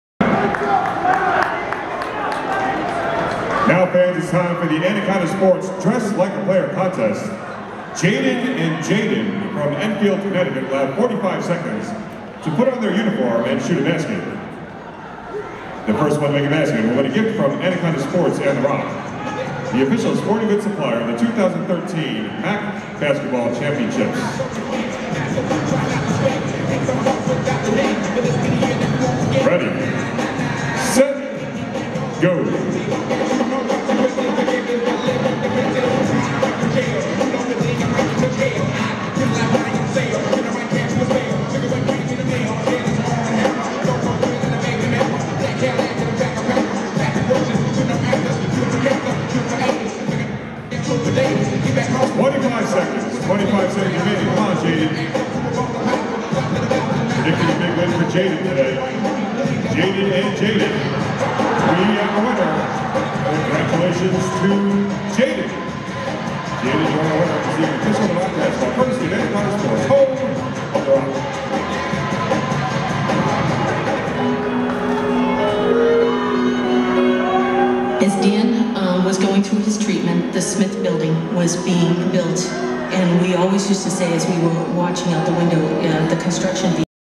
Dress Like a Player Contest - Iona vs Niagara Men's Basketball - MAAC Tournament - March 10, 2013
Dress Like a Player Contest During 2nd Half Timeout
Iona Gaels vs Niagara Purple Eagles - Men's Basketball
MAAC Conference Tournament - Semi-Final
MassMutual Center  -  Springfield, Massachusetts